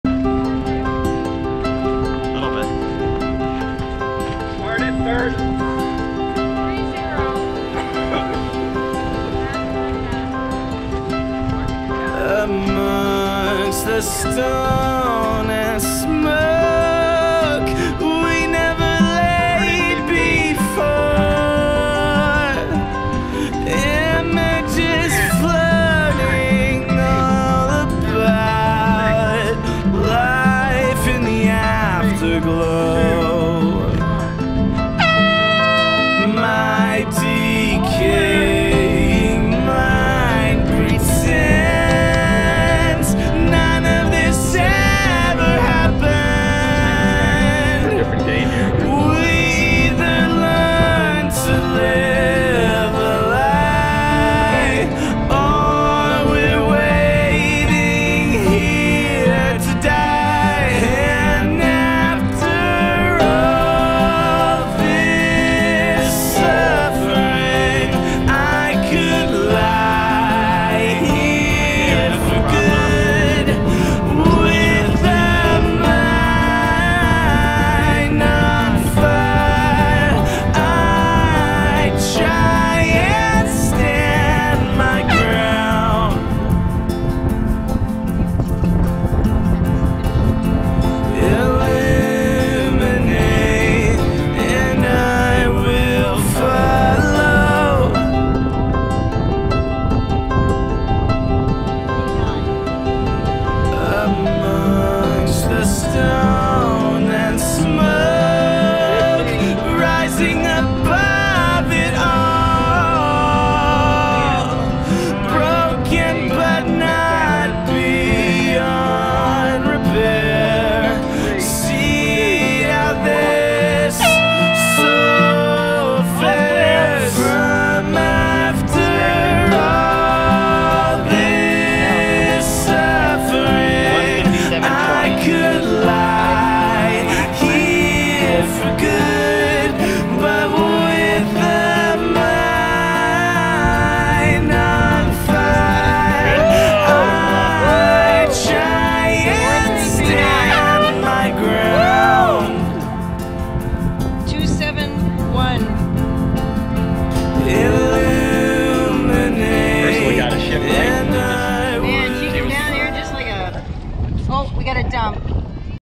Labor Day sunfish race 2 and 3
2019 Labor Day Regatta Sunfish races 2 and 3
I claim no copyrights on the music in the video
Song by the Dear Hunter